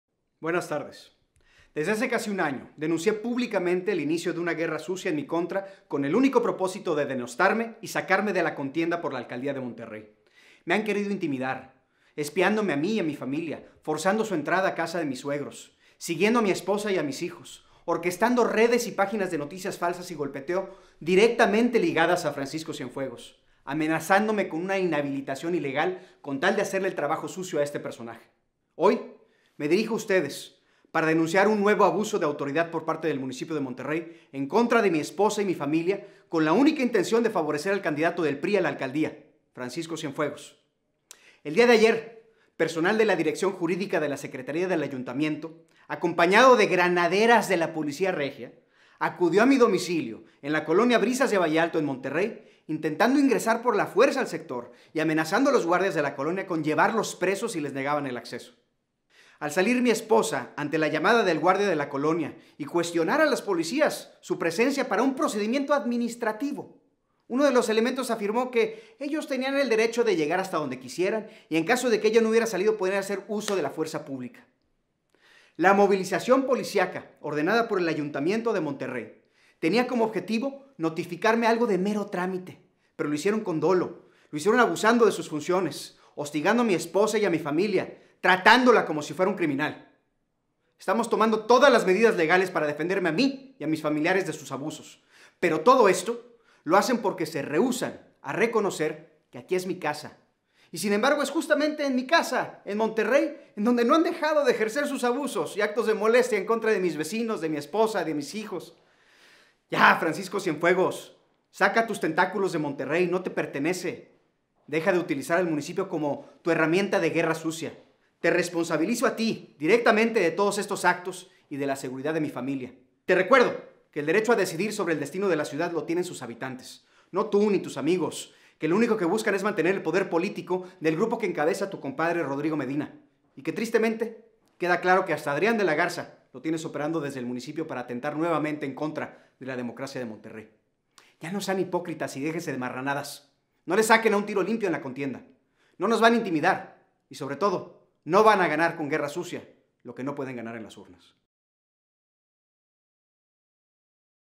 No Nos Van A Intimidar
Hoy me dirijo a ustedes para denunciar un nuevo abuso de autoridad por parte del Municipio de Monterrey en contra de mi esposa y mi familia con la única intención de favorecer al candidato del PRI a la Alcaldía, Francisco Cienfuegos.
Ya no sean hipócritas y déjense de marranadas, no le saquen a un tiro limpio en la contienda. No nos van a intimidar y sobre todo, no van a ganar con guerra sucia lo que no pueden ganar en las urnas.